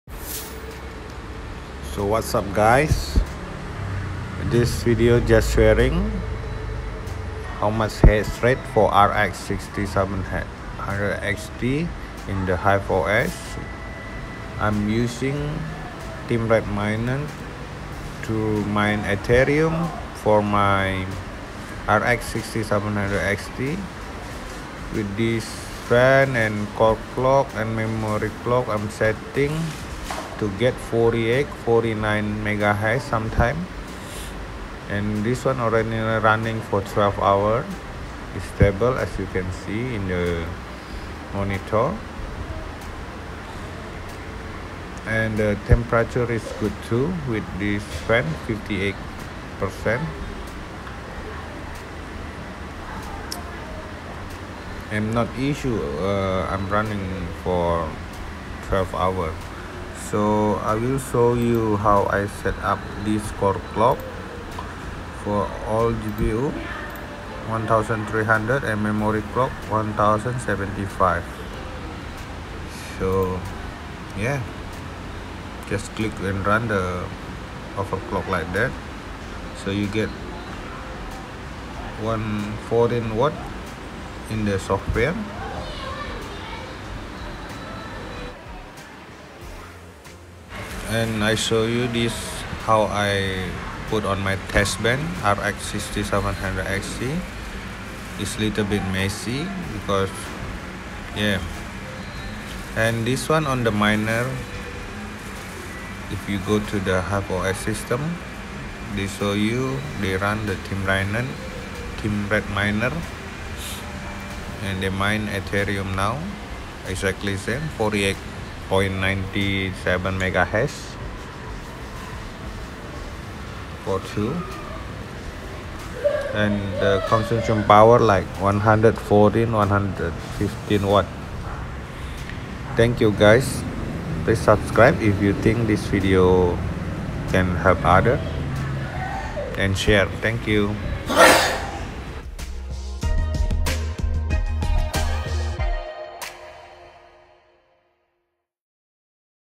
Hasrate RX6700XT : 48/49 Mhs mining Ethereum in Hive os with Temredminer
Note : please do you own research before follow instructions in this video.
My video just for sharing.
All risk can be happen when we overclok and undervolt gpu, and that our own risk.